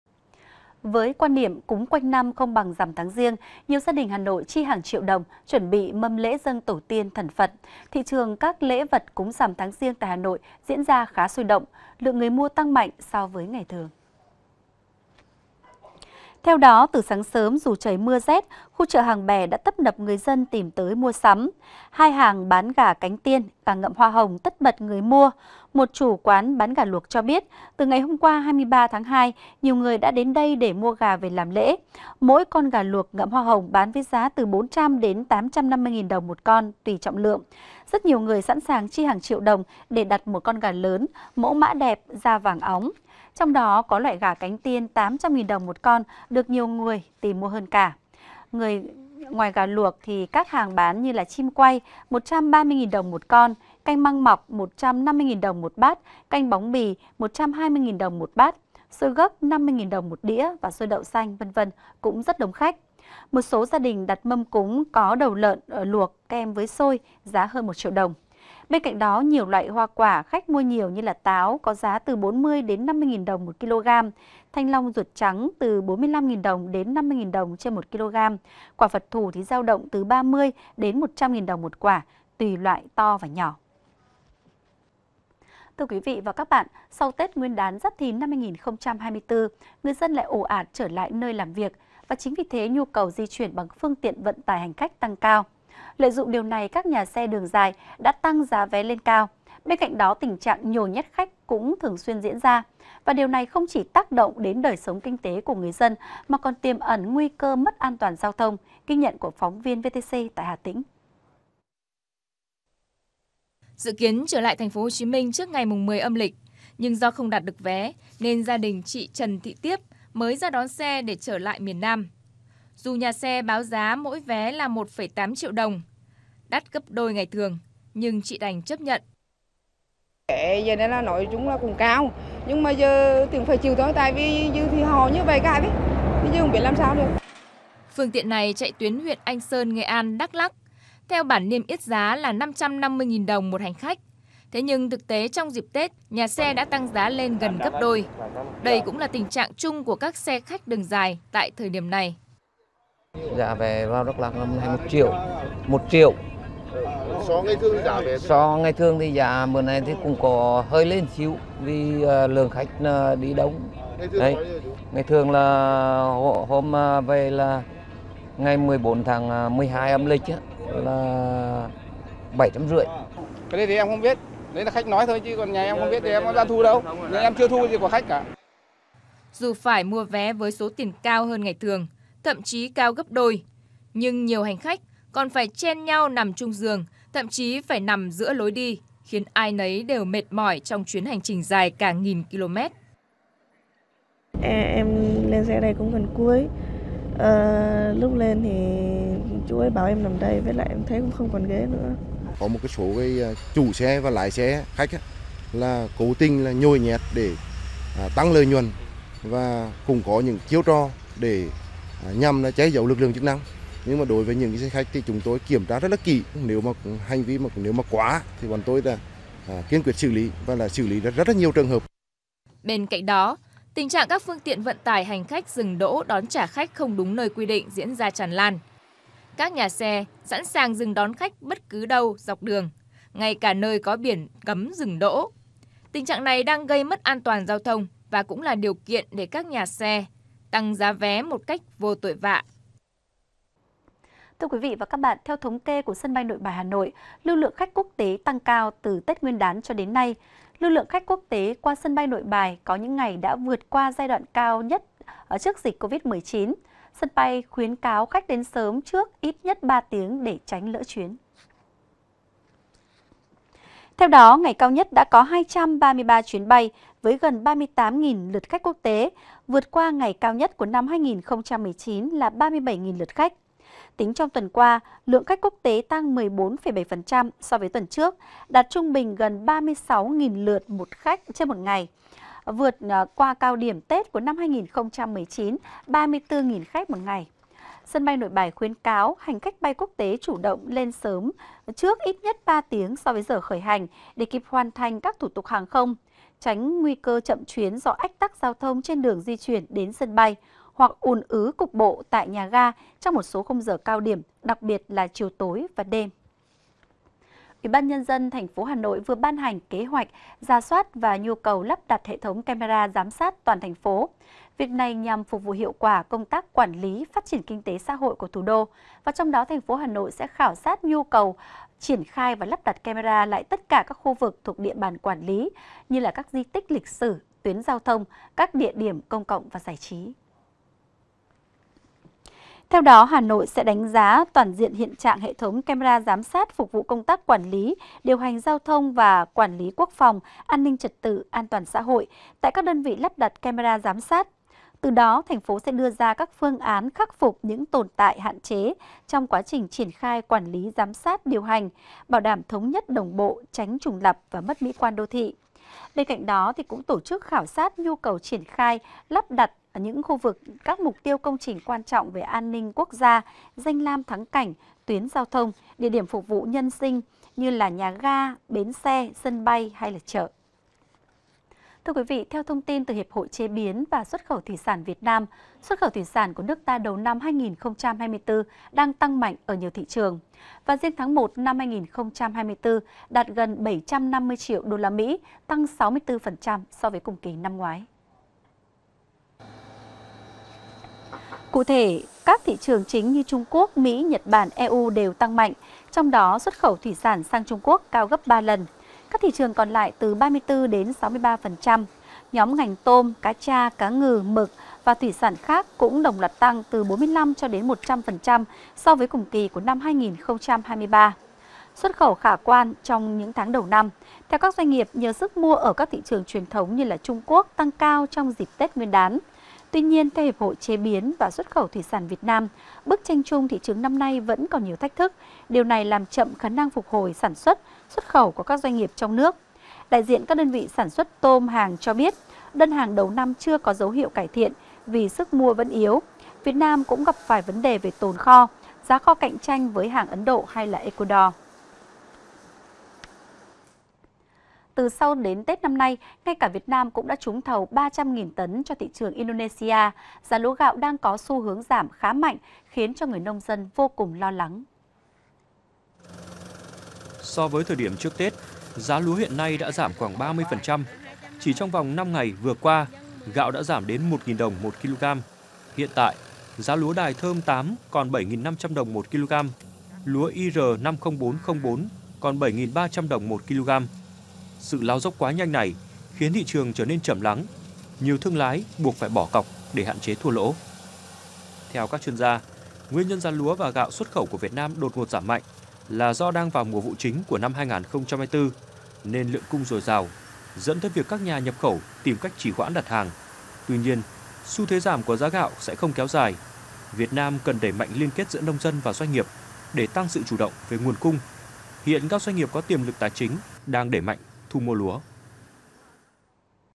Với quan niệm “cúng quanh năm không bằng ngày rằm tháng giêng”: Nhiều gia đình chi lớn để sắm lễ
Người Việt thường nghe câu: Lễ Phật quanh năm không bằng ngày rằm tháng giêng. Ngày rằm tháng giêng (15 tháng giêng âm lịch) vì sao lại quan trọng trong tâm thức người Việt như vậy, chuẩn bị mâm cúng thế nào?

Người Việt vốn coi trọng sự khởi đầu với niềm tin rằng "đầu xuôi đuôi lọt" nên ngày đầu năm hay điều gì bắt đầu rất được coi trọng; và rằm tháng giêng - ngày rằm đầu tiên của năm mới cũng vậy.

Vào ngày này, Phật tử, đông đảo mọi người hay đến chùa cầu một năm mới bình an, suôn sẻ cho đại gia đình, mọi người xung quanh.